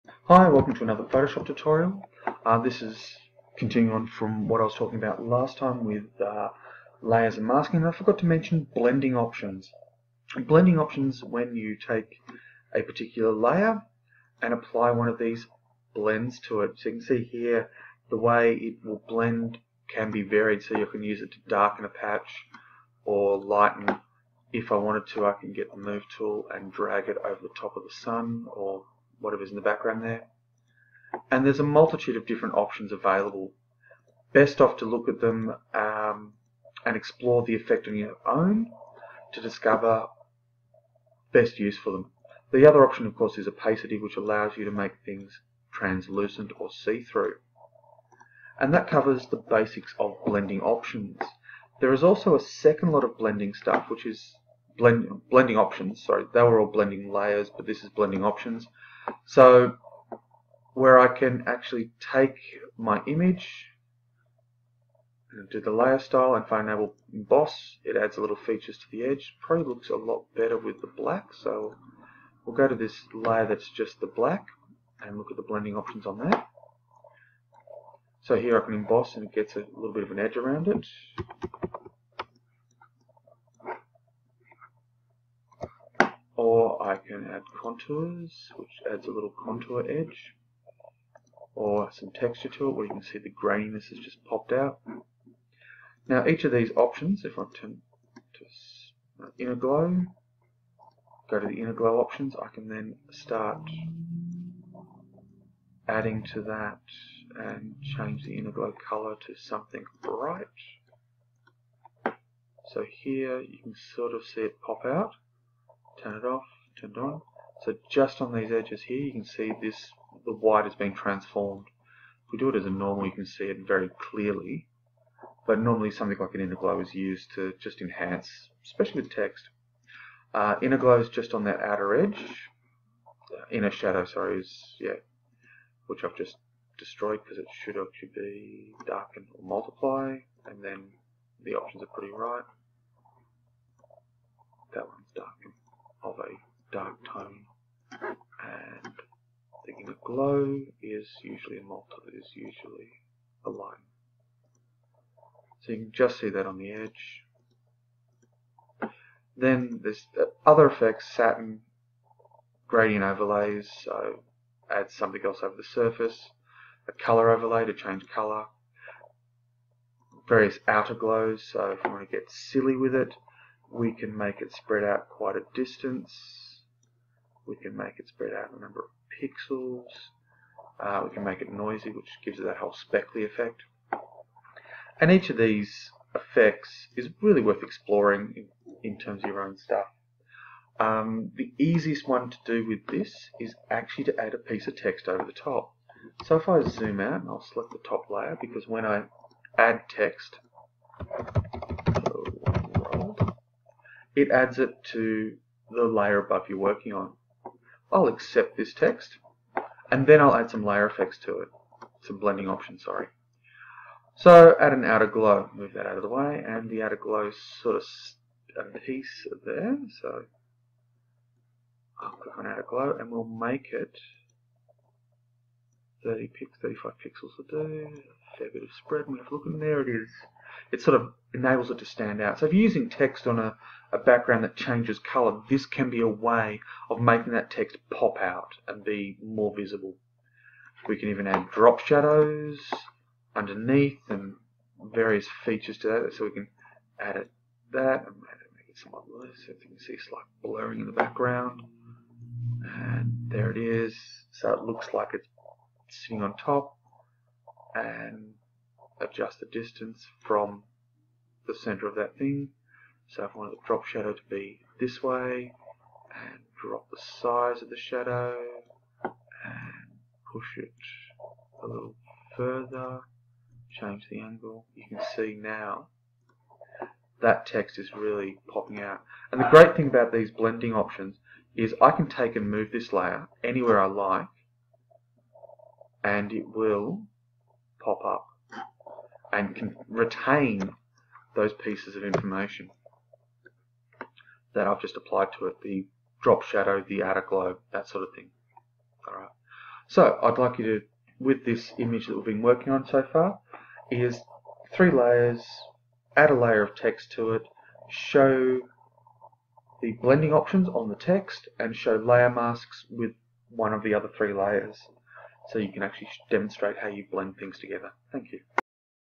Blending Options for Layers in Photoshop CC 2017
This tutorial covers how to use Blending options with layers to create interesting effects.